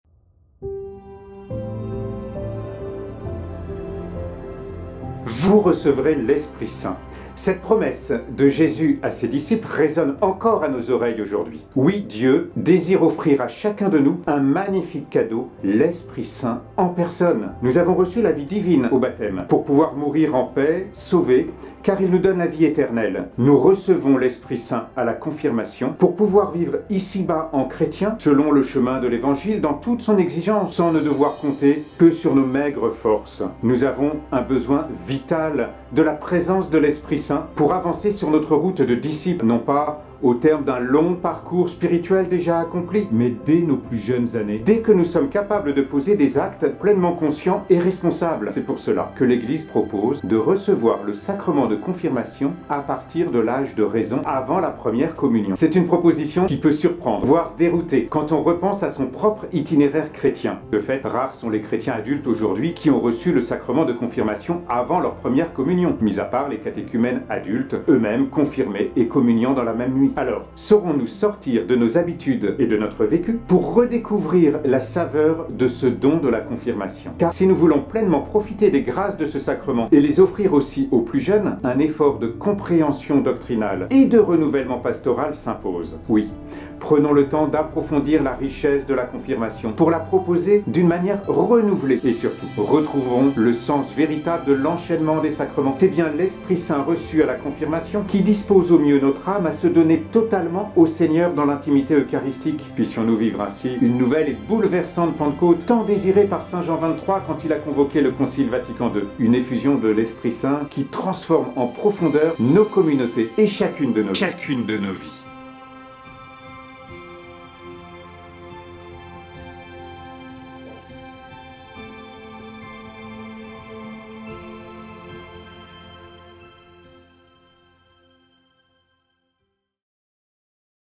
La Confirmation à sa juste place
Présentation du livre de l'abbé François Dedieu "La Confirmation à sa juste place - Plaidoyer pour proposer ce sacrement avant la première communion", aux éditions Artège (24 janvier 2024).